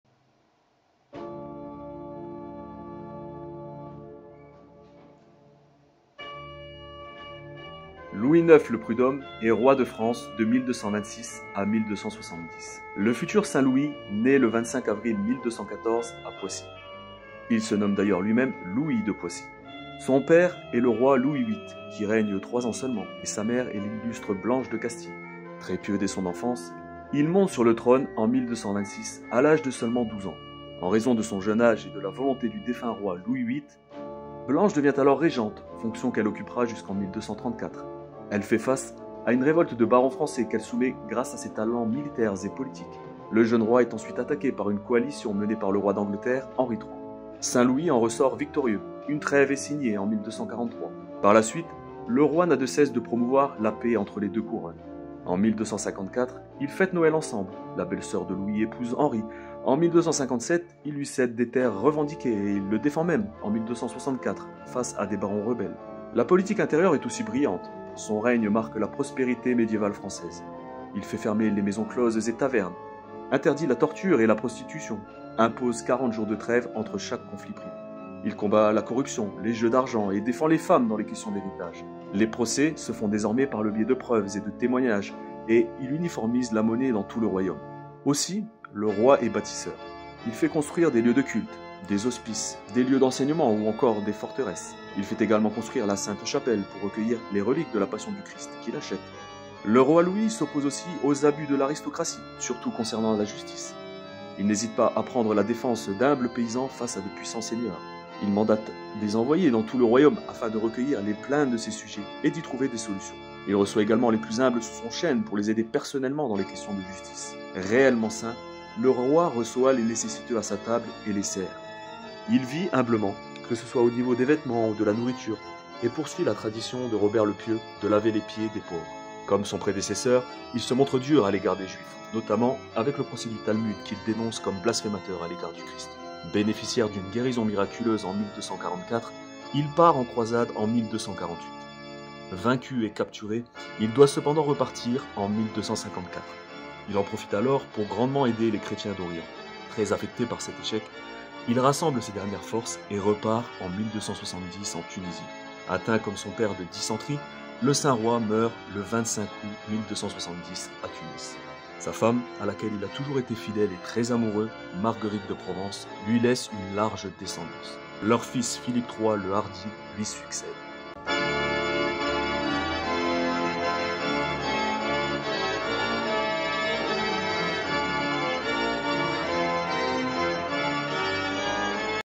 Louis IX le Prudhomme (1226 - 1270) - Présentations des Rois de France
Saint Louis…. Tant de choses à dire sur celui qui est possiblement le plus grand Roi de France, il en est certainement le plus saint tout du moins. Bon militaire, artisan de paix, homme profondément charitable et humble, croisé, réformateur, justicier… Il est certain qu’une vidéo de présentation est insuffisante pour tout dire à son sujet, n’hésitez pas à nous enrichir de vos anecdotes et informations supplémentaires en commentaires.

Illustrations :

- Introduction officielle du CAL.
- Drapeau blanc.
- Portrait de Louis IX - Auguste de Creuse – 1837.
- Louis VIII, dit le Lion, roi de France en 1223 - Henri Lehmann – 1837.
- Blanche de Castille , reine de France - Antoine Louis François Sergent – 1791.
- Carte du royaume.
- Saint Louis confiant la régence à sa mère, Blanche de Castille - Joseph-Marie Vien - XVIIIe siècle.
- Blanche fait rompre les prisons du Chapitre de Paris - Antoine Louis François Sergent – 1791.
- Henri III d'Angleterre - Matthieu de Paris - Vers 1255.
- Saint Louis pardonnant à Pierre de Bretagne - Georges Rouget – 1817.
- Autre portraict de S Lovys - Jacques de Bie – 1634.
- Saint Louis médiateur entre le roi d'Angleterre et ses barons - Georges Rouget – 1820.
- Statue de Saint Louis dans la Sainte-Chapelle.
- Le sacre de Saint Louis - Vers 1729.
- Louys IXe surnommé St - Nicolas II de Larmessin – 1690.
- Divus Ludovicus - Jean du Tillet – 1580.jpg.
- Statue de St Louis et de Marguerite de Provence - Vers 1729.
- Intérieur de la Sainte-Chapelle.
- Saint Louis, Roi de France, recevant les ambassadeurs du Prince des Assassins - Guy-Nicolas Brenet - XIXe siècle.
- Saint Louis rendant la justice sous le chêne de Vincennes - Georges Rouget – 1826.
- Saint Louis rendant la justice sous le chêne de Vincennes - Pierre-Narcisse Guérin – 1816.
- Saint Charlemagne et saint Louis - Grégoire Huret - XVIIe siècle.
- Sainct Louis IX de ce nom Roy de France - Jacques de Bie – 1634.
- Louis IX, dit Saint Louis, Roi de France - Émile Signol – 1844.
- Saint Louis fait prisonnier par les Sarrasins - Gustave Doré - XIXe siècle.
- Louis IX dit St Louis roi de France - Antoine Louis François Sergent – 1786.
- Le Roy Sainct Lois - Jean du Tillet – 1566.
- Marguerite de Provence, reine de France - Antoine Louis François Sergent – 1791.
- Philippe III le Hardi, roi de France - Alexandre Laemlein – 1844.
- Armes du Roi de France.

Montage et miniature effectués par la section de Lille et voix de Maxime du CAL-Dunkerque.

Merci du visionnage.